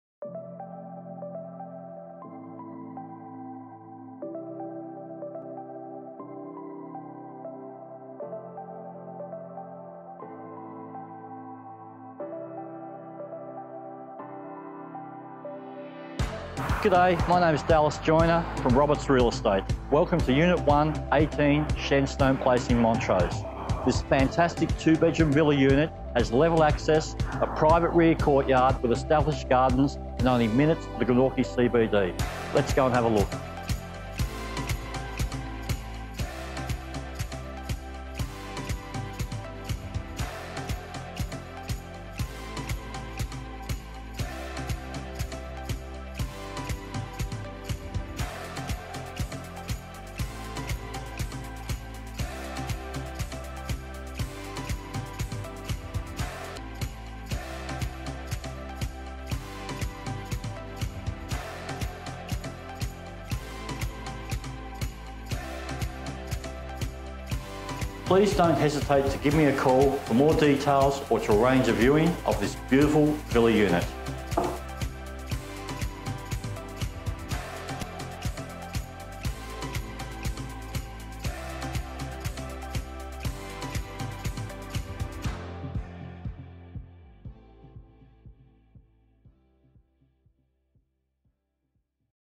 1/18 Shenstone Place Montrose, Dallas Joiner, Roberts Real Estate Tasmania
1/18 Shenstone Place Montrose as presented by Dallas Joiner of Roberts Real Estate Tasmania